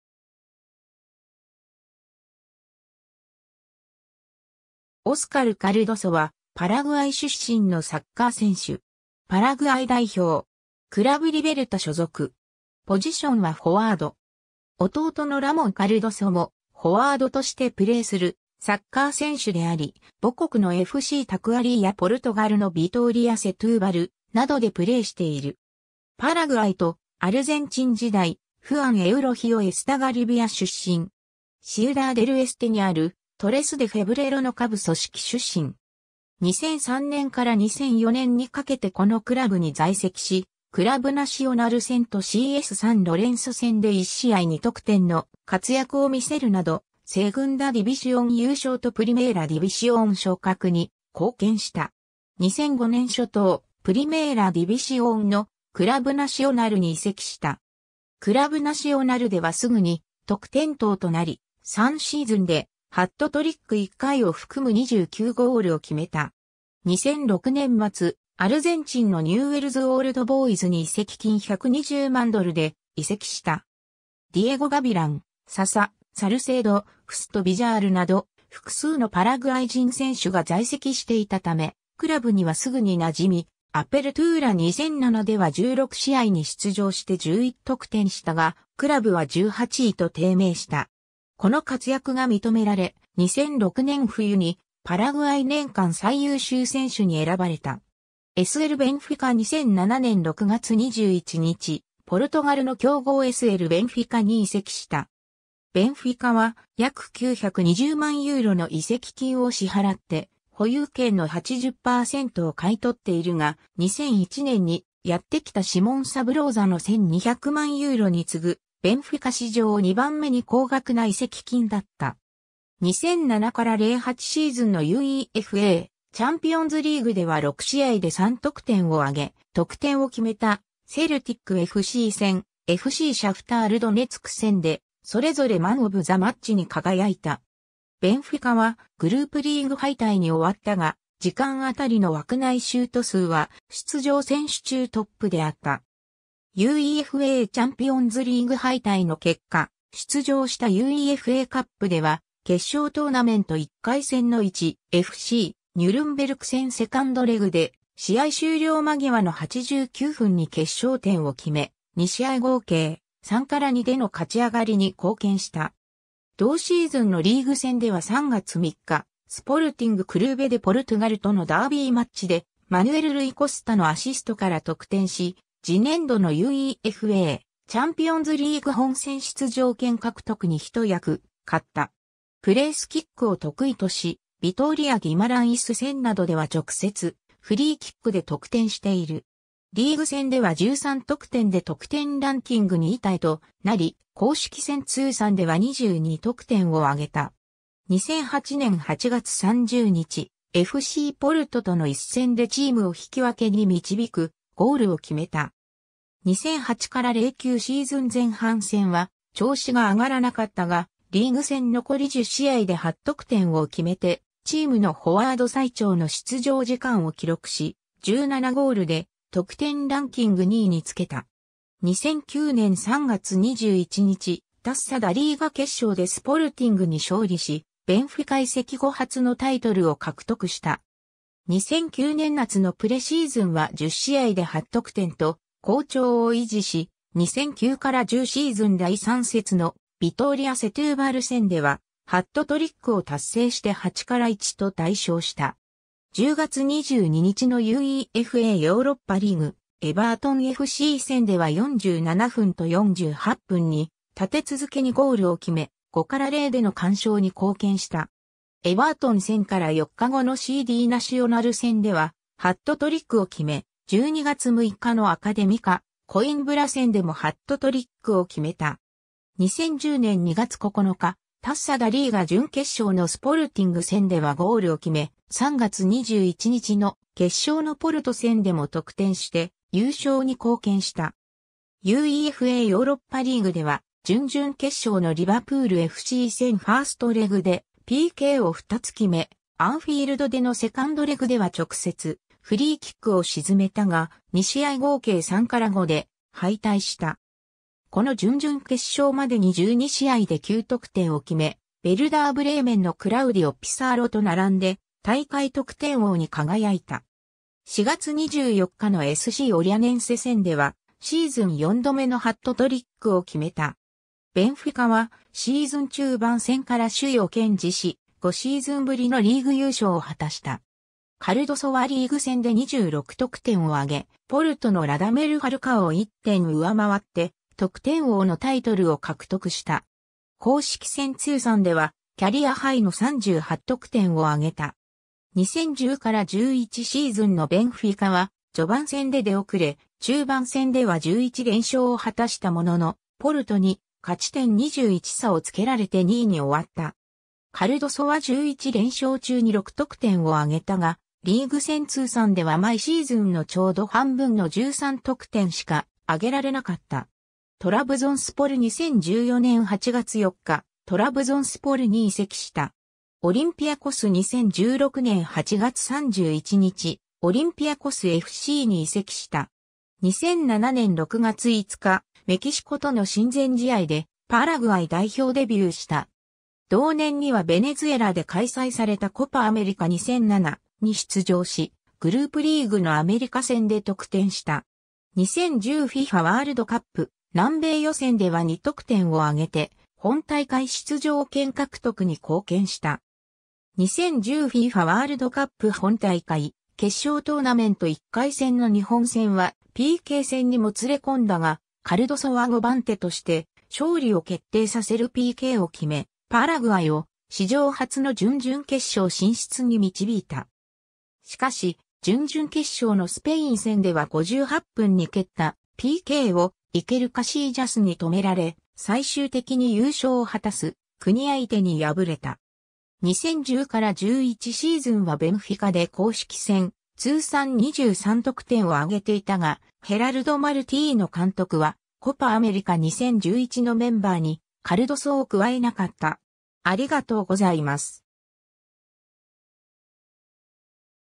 オスカル・カルドソ
クラブ・ナシオナルの選手
ニューウェルズ・オールドボーイズの選手
トラブゾンスポル・クラブ_(サッカー)_の選手
クラブ・リベルタの選手
オスカル・カルドソ（Óscar Cardozo, 1983年5月20日 - ）はパラグアイ出身のサッカー選手。
パラグアイ代表。
クラブ・リベルタ所属。
弟のラモン・カルドソもフォワードとしてプレーするサッカー選手であり、母国のFCタクアリーやポルトガルのヴィトーリア・セトゥーバルなどでプレーしている。
 パラグアイとアルゼンチン時代 フアン・エウロヒオ・エスタガリビア出身。
シウダー・デル・エステにあるトレス・デ・フェブレロの下部組織出身。
2003年から2004年にかけてこのクラブに在籍し、クラブ・ナシオナル戦とCSサン・ロレンソ戦で1試合2得点の活躍を見せるなど、セグンダ・ディビシオン（2部）優勝とプリメーラ・ディビシオン（1部）昇格に貢献した。
2005年初頭、プリメーラ・ディビシオンのクラブ・ナシオナルに移籍した。
クラブ・ナシオナルではすぐに得点頭となり、3シーズンでハットトリック1回を含む29ゴールを決めた。
 2006年末、アルゼンチンのニューウェルズ・オールドボーイズに移籍金120万ドルで移籍した。
ディエゴ・ガビラン、ササ・サルセード、フスト・ビジャールなど複数のパラグアイ人選手が在籍していたためクラブにはすぐに馴染み、アペルトゥーラ2007では16試合に出場して11得点したが、クラブは18位と低迷した。
この活躍が認められ、2006年冬にパラグアイ年間最優秀選手に選ばれた。
 SLベンフィカ 2007年6月21日、ポルトガルの強豪SLベンフィカに移籍した。
ベンフィカは約920万ユーロの移籍金を支払って保有権の80%を買い取っているが、2001年にやってきたシモン・サブローザの1200万ユーロに次ぐ、ベンフィカ史上2番目に高額な移籍金だった。
2007-08シーズンのUEFAチャンピオンズリーグでは6試合で3得点を挙げ、得点を決めたセルティックFC戦・FCシャフタール・ドネツク戦でそれぞれマン・オブ・ザ・マッチに輝いた。
ベンフィカはグループリーグ敗退に終わったが、時間当たりの枠内シュート数は出場選手中トップであった。
UEFAチャンピオンズリーグ敗退の結果出場したUEFAカップでは、決勝トーナメント1回戦の1.FCニュルンベルク戦セカンドレグ (2-2) で試合終了間際の89分に決勝点を決め、2試合合計3-2での勝ち上がりに貢献した。
同シーズンのリーグ戦では3月3日、スポルティング・クルーベ・デ・ポルトゥガルとのダービーマッチでマヌエル・ルイ・コスタのアシストから得点し、次年度のUEFAチャンピオンズリーグ本戦出場権獲得に一役買った。
プレースキックを得意とし、ヴィトーリア・ギマランイス戦などでは直接フリーキックで得点している。
リーグ戦では13得点で得点ランキング2位タイとなり、公式戦通算では22得点を挙げた。
2008年8月30日、FCポルトとの一戦 (1-1) でチームを引き分けに導くゴールを決めた。
2008-09シーズン前半戦は調子が上がらなかったが、リーグ戦残り10試合で8得点を決めてチームのフォワード最長の出場時間を記録し、17ゴールで得点ランキング2位につけた。
2009年3月21日、タッサ・ダ・リーガ決勝でスポルティングに勝利し、ベンフィカ移籍後初のタイトルを獲得した。
 2009年夏のプレシーズンは10試合で8得点と好調を維持し、2009-10シーズン第3節のヴィトーリア・セトゥーバル戦ではハットトリックを達成して8-1と大勝した。
10月22日のUEFAヨーロッパリーグ・エヴァートンFC戦では47分と48分に立て続けにゴールを決め、5-0での完勝に貢献した。
エヴァートン戦から4日後のCDナシオナル戦 (6-1) ではハットトリックを決め、12月6日のアカデミカ・コインブラ戦 (4-0) でもハットトリックを決めた。
2010年2月9日、タッサ・ダ・リーガ準決勝のスポルティング戦ではゴールを決め、3月21日の決勝のポルト戦でも得点して優勝に貢献した。
UEFAヨーロッパリーグでは、準々決勝のリヴァプールFC戦ファーストレグ (2-1) でPKをふたつ決...